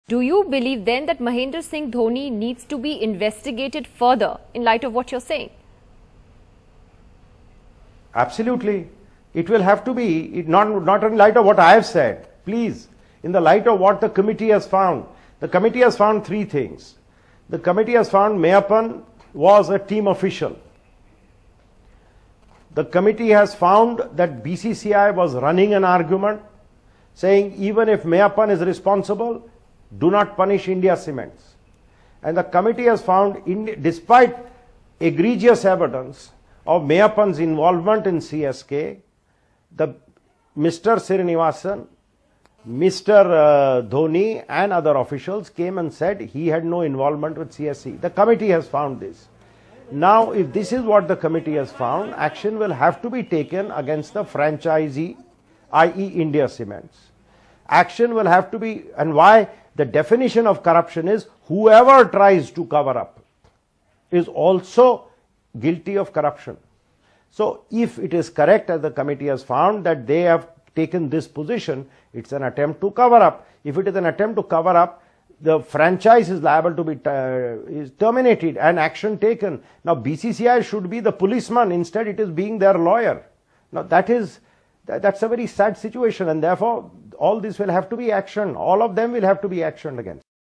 IPL scandal: Investigate MS Dhoni, says top lawyer Harish Salve
Senior lawyer Harish Salve, who accused Chennai Super Kings captain MS Dhoni of lying to the probe committee on the betting scandal, tells NDTV that the Team India skipper needs to be properly investigated and calls for a radical overhaul of the BCCI. The Supreme Court has temporarily removed N. Srinivasan as BCCI president.